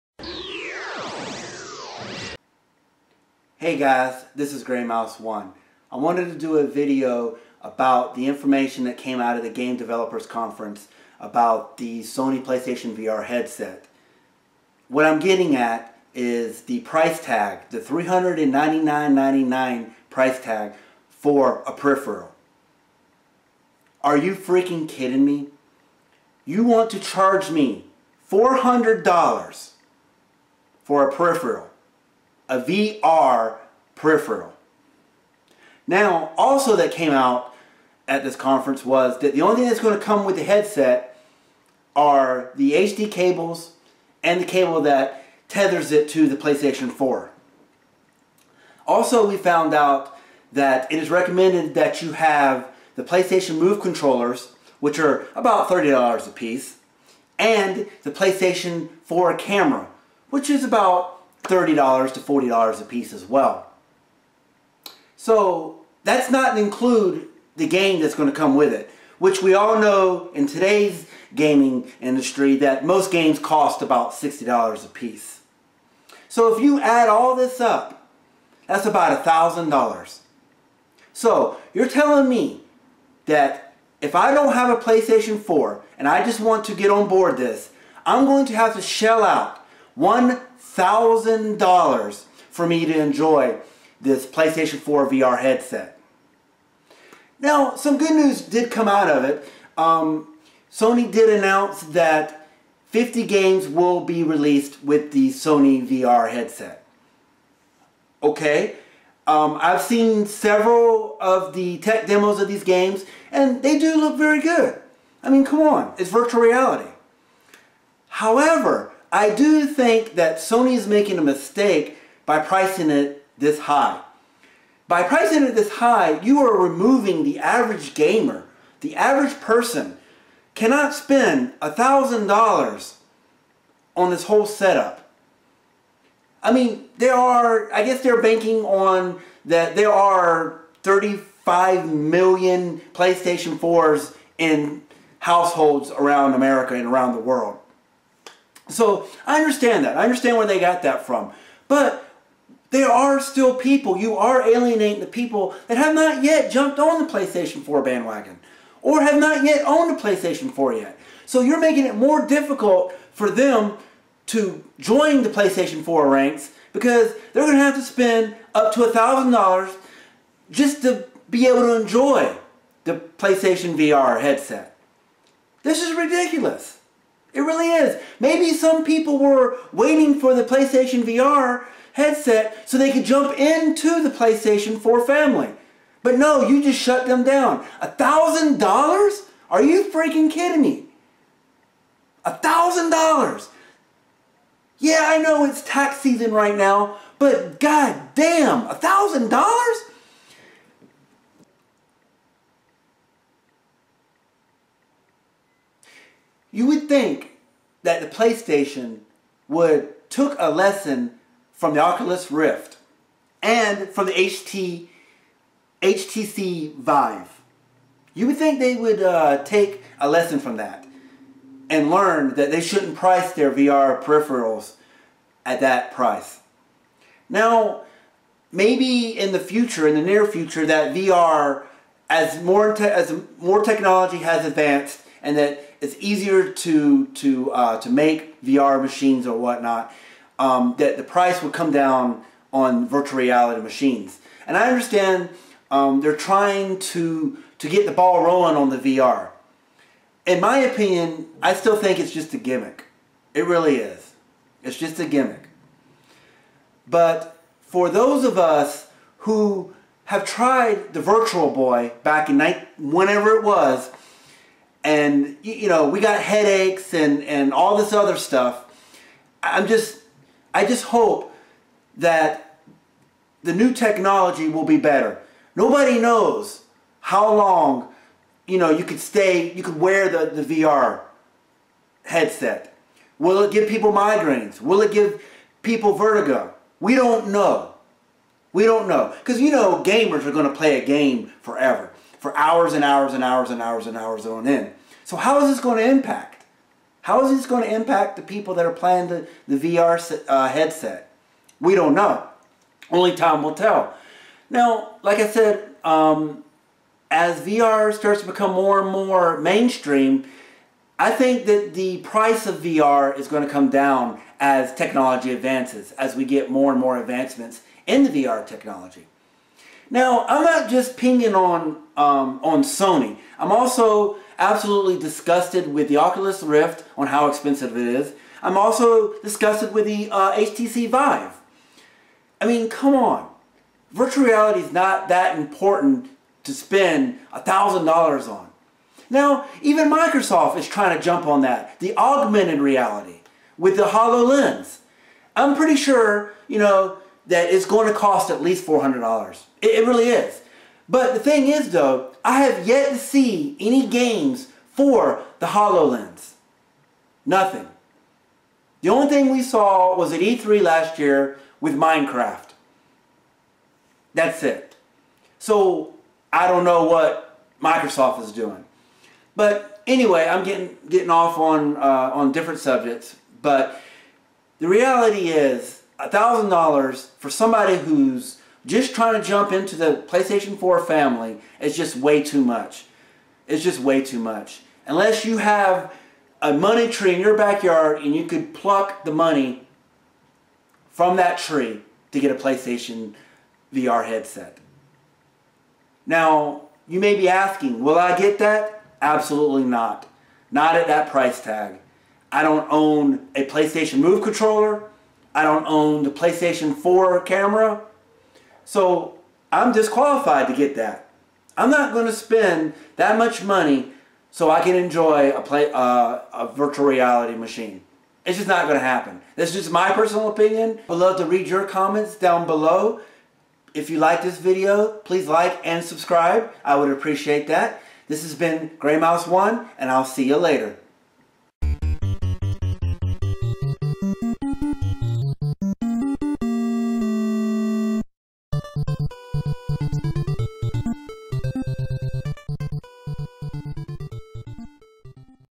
Sony Playstation VR Headset priced at $399.99
In this video I discuss the price tag of the Sony PSVR headset. When you buy the headset it only comes with a HDMI cable and a tether that connects to your PS4. If you don't have a PS camera you are going to have to buy a PS 4 Camera. If you don't have the PS4 you are going to have to buy one at $350-$400 if you buy the bundles. This doesn't even include the $60 VR game either. Are you going to buy the PSVR headset?